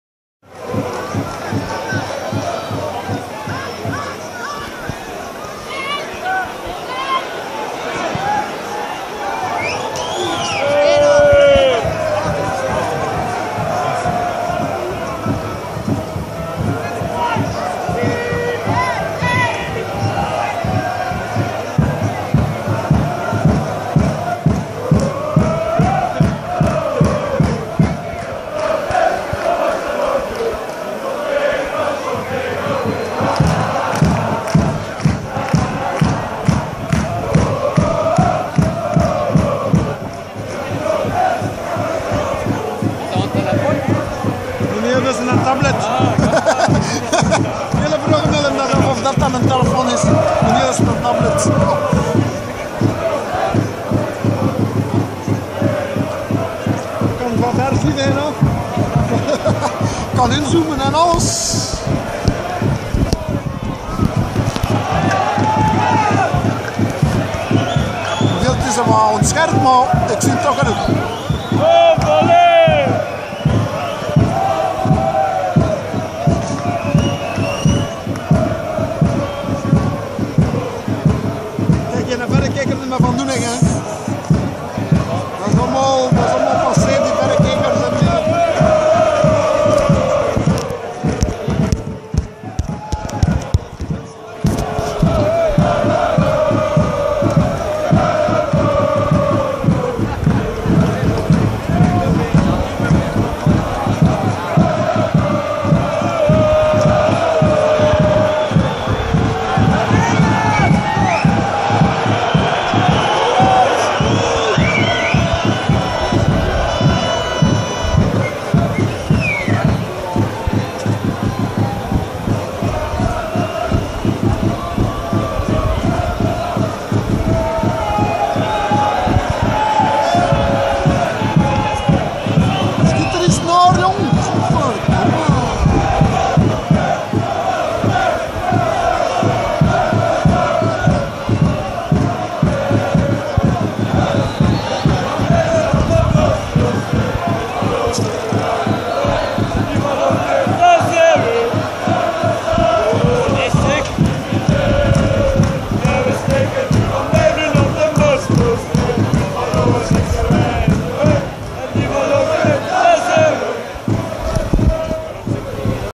Stvv komt op daknam een punt halen
Net voor de vriendschappelijke wedstrijden van de red devils. speelde stvv op  daknam en  graaide er op het  allerlaatste moment nog  een puntje mee terug naar  sint -truiden